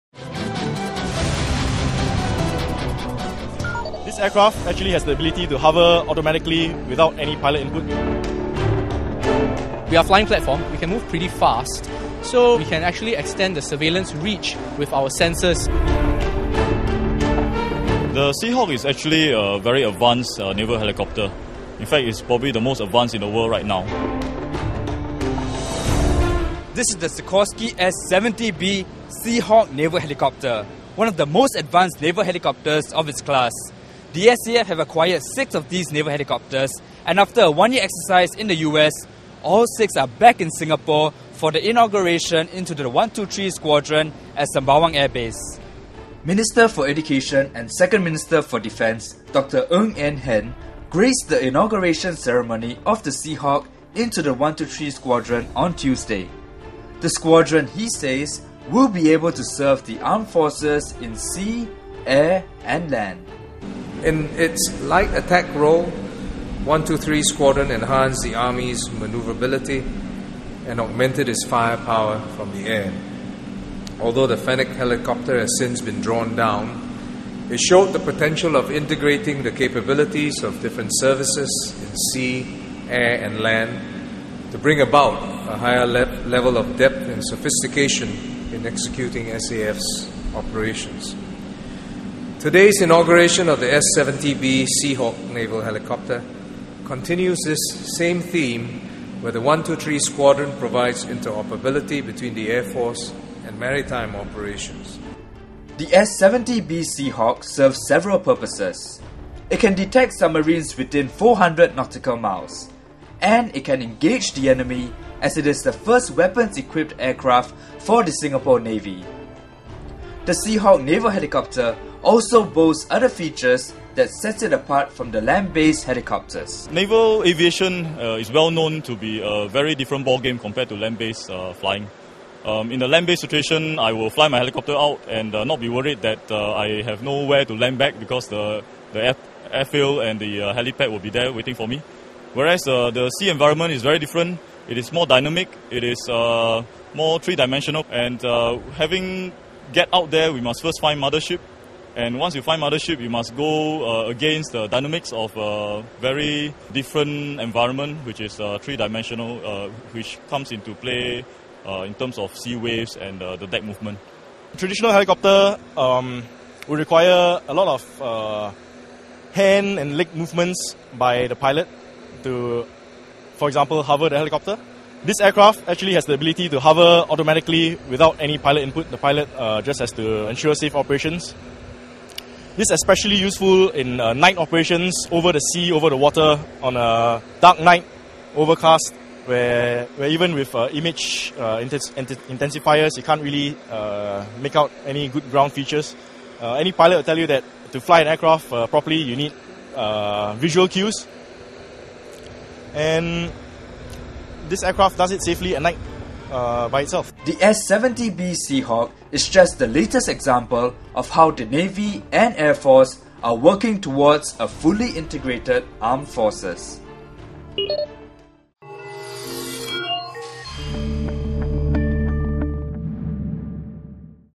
SAF's new "Terror of the Seas"
The most advanced naval helicopter of its class, the S-70B Seahawk has joined the 123 squadron. Find out why this helicopter is so highly regarded by the SAF.